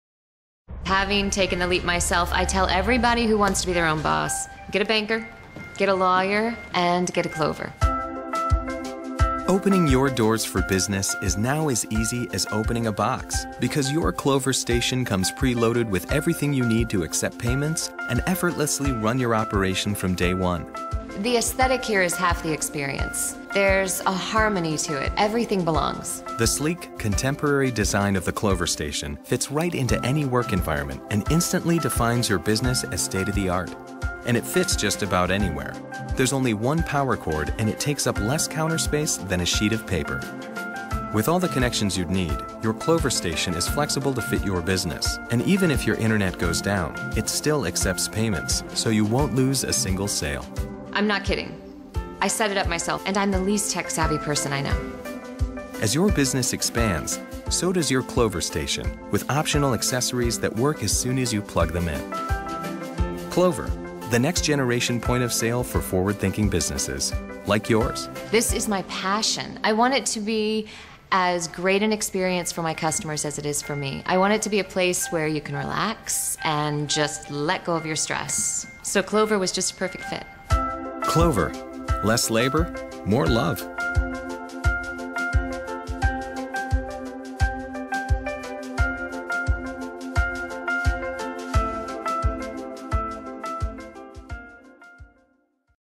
Meet Clover
Clover is an all-in-one, payment processing solution for your small business. What's more, this thing is powerful. Ring up sales, sure, but also view your inventory, track revenues, manage employees, view reports and take advantage of a ton of applications that are customized for your business.